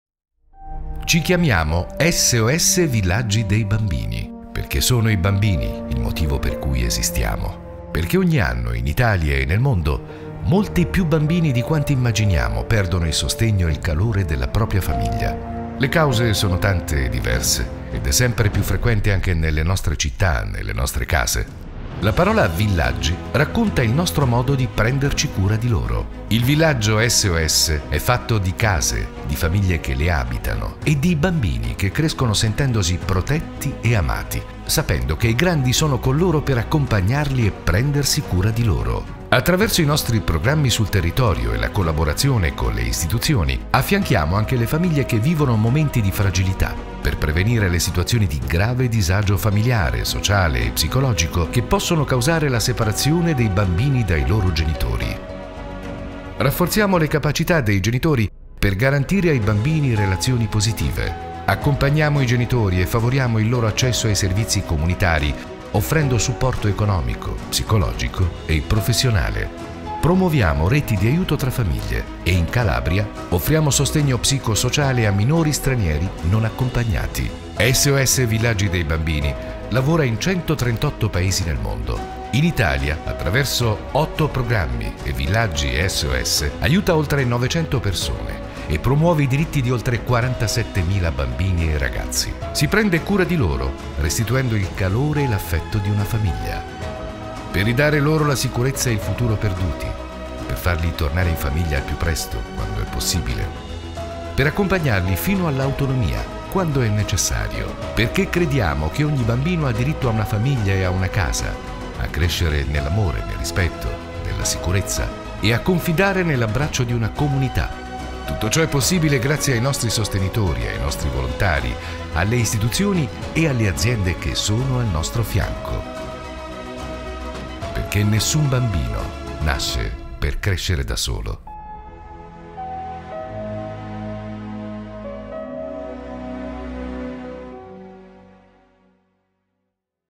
Perché ci chiamiamo SOS Villaggi dei Bambini?
Perché vogliamo garantire a ogni bambino il diritto di crescere sereno e in salute in un ambiente familiare accogliente, e di sviluppare pienamente le sue potenzialità. Scopri di più visitando il nostro sito web